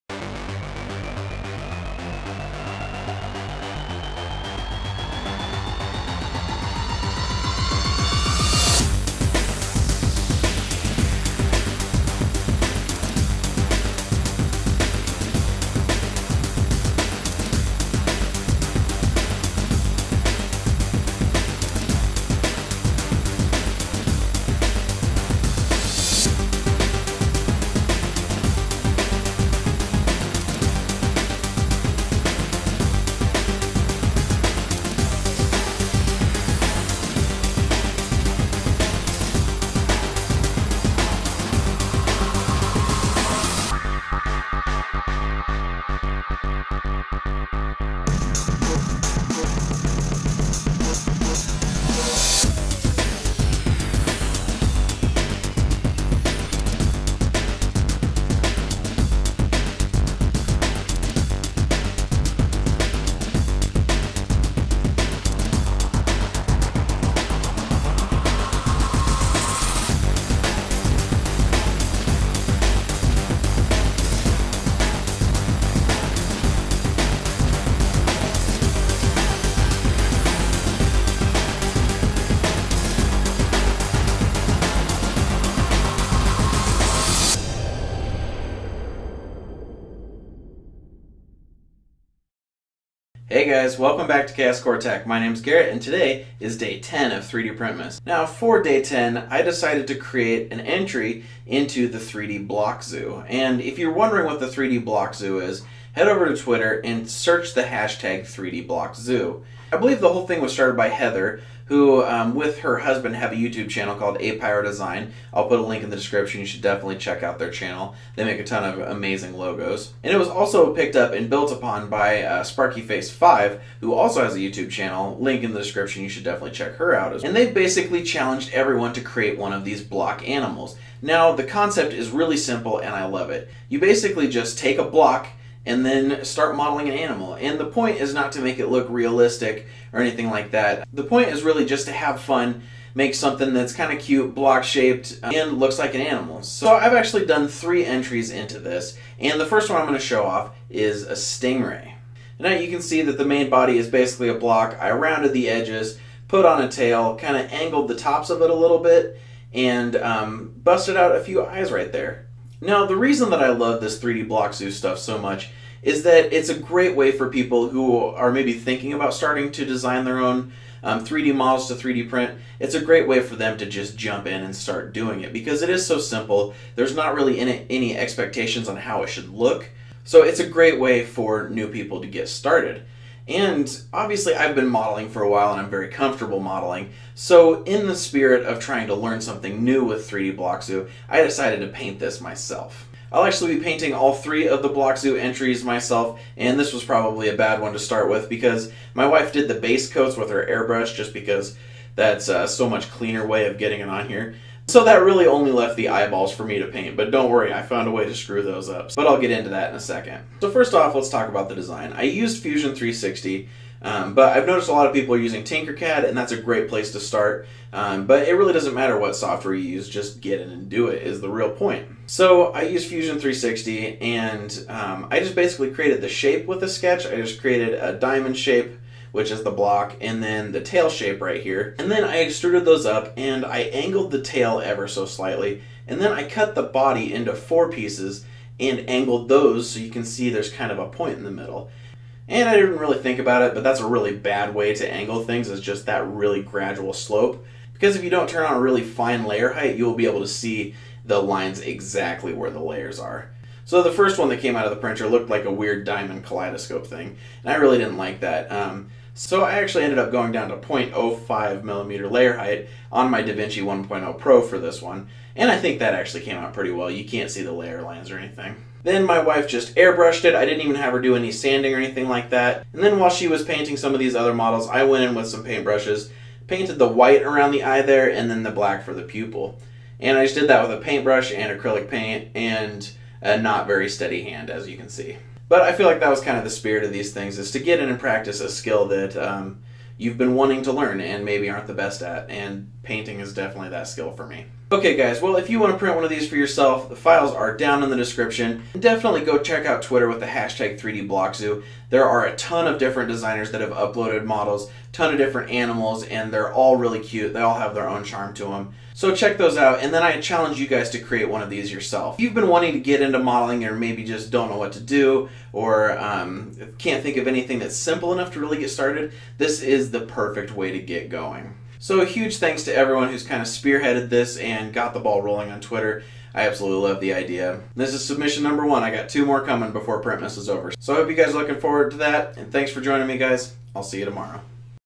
3DBlockZoo Stingray - 3DPrintmas Day 10
That's right! We're on day 10 of printmas! And today I'm adding to the 3DBlockZoo.

I created a block stingray. I created this in Fusion 360 by sketching out a diamond and the tail using splines. Then I added in a few chamfers and fillets before adding a couple eyes with spheres.

Then I printed it out on my Da Vinci 1.0 Pro. I actually had to push it to 0.05mm layer height to make it so the slight slope of the body wasn't really jagged looking.

Then I handed it off to my wife who spray painted the main color of the body on and then I painted the eyes one with a paintbrush.

If you create something for the block zoo don't forget to tweet it with the hashtag 3DBlockZoo!

Print Settings:
Printer: Da Vinci 1.0 Pro
Filament: Hatchbox PLA
Layer Height: 0.05mm
Infill: 30%
Supports: No
Raft: No